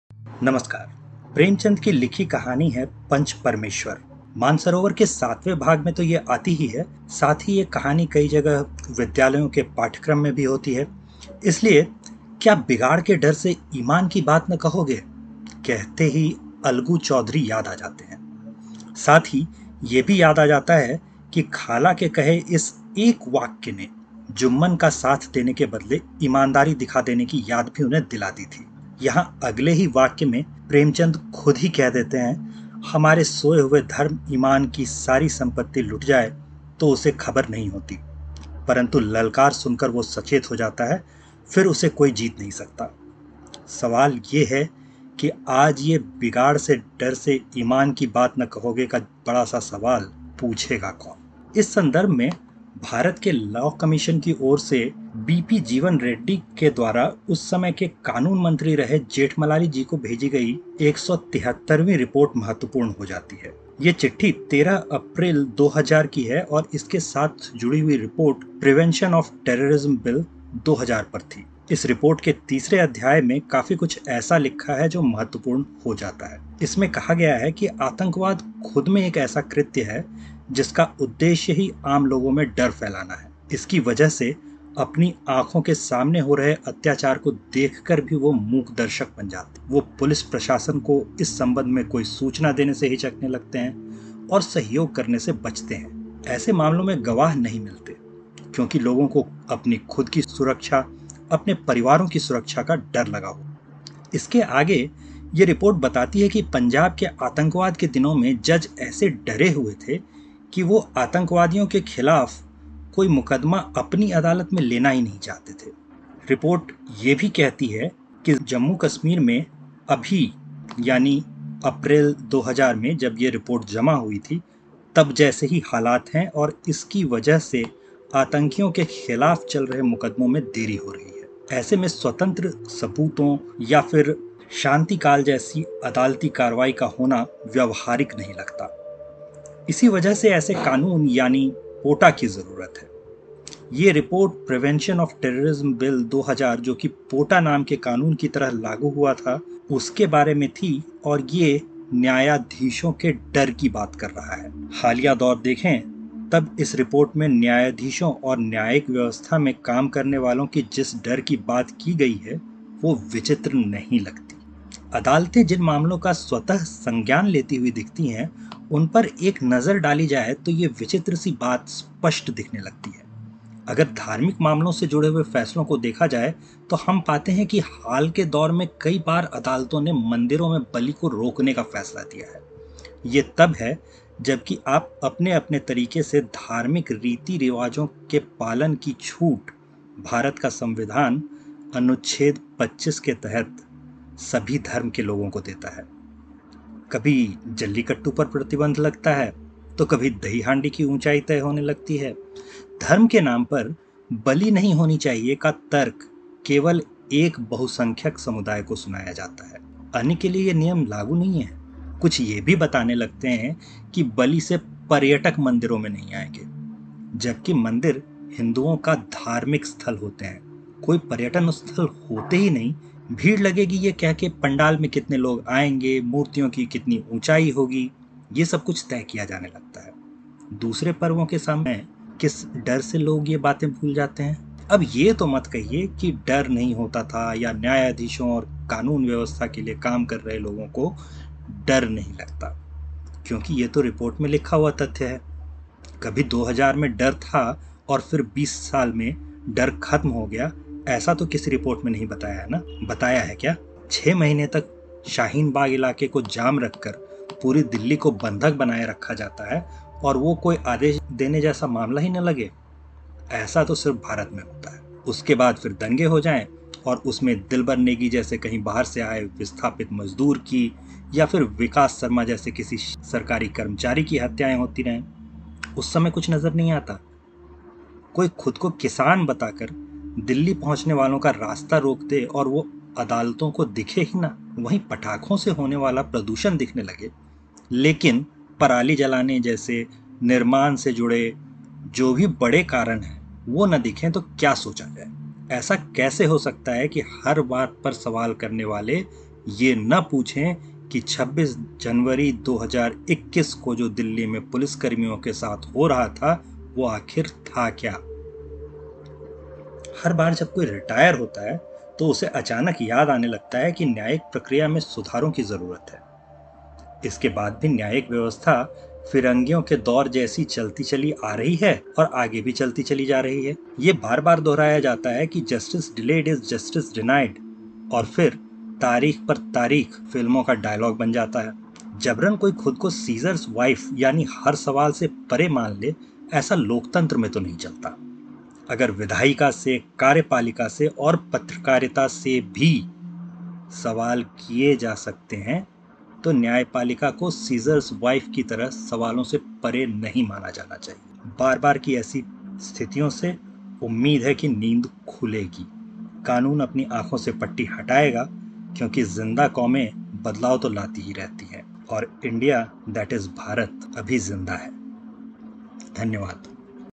Second Grade Citizens of India and the Courts
Are the Indian courts ever sympathetic towards anything that's even remotely associated to Hindus? Let's take a look!

To access the report that has been referred in the video